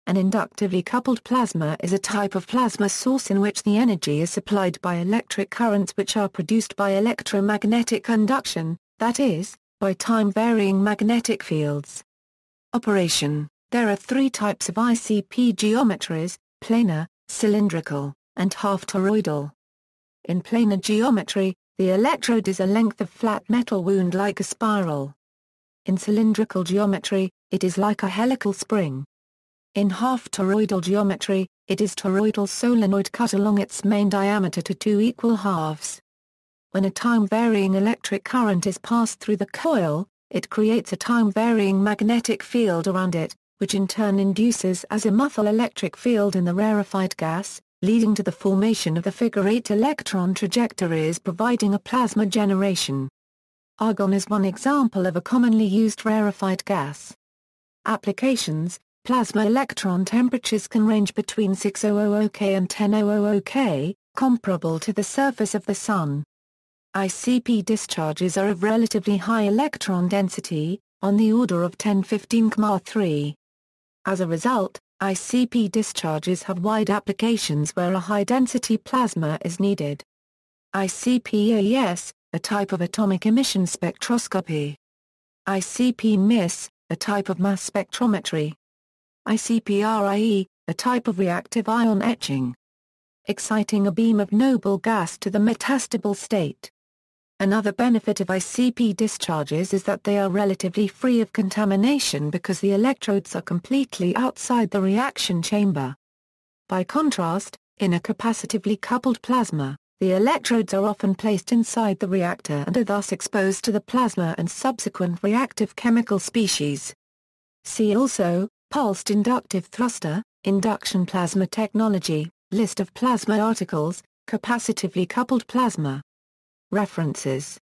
Inductively coupled plasma
An inductively coupled plasma is a type of plasma source in which the energy is supplied by electric currents which are produced by electromagnetic induction, that is, by time-varying magnetic fields.